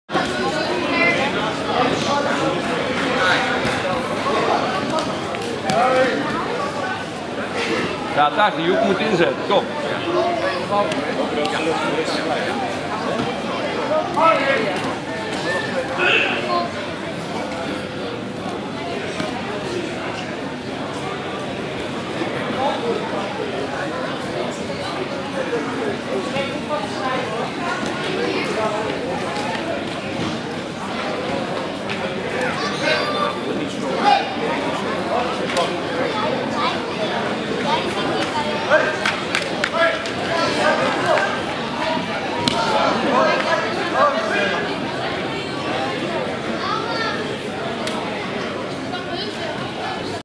Prinsenbeek 200311-1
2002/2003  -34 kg
First match: opponent is not strong, tai otoshi and the job is done.